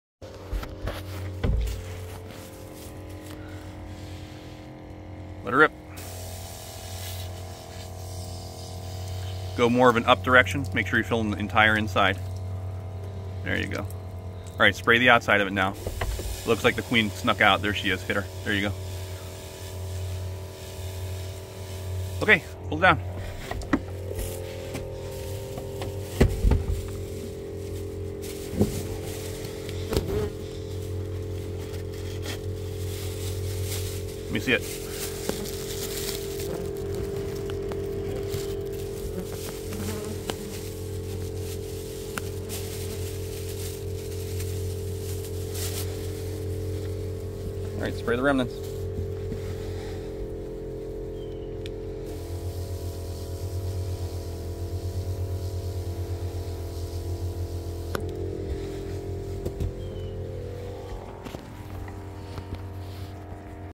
Small Yellow Jacket Nest Under a Bar
Yellow jacket nests are starting to get big enough to notice. These early season nests aren't as big as some of the monsters we sometimes get, but they'll cause their share of damage, especially if you sit at this bar on the customer's back deck without seeing them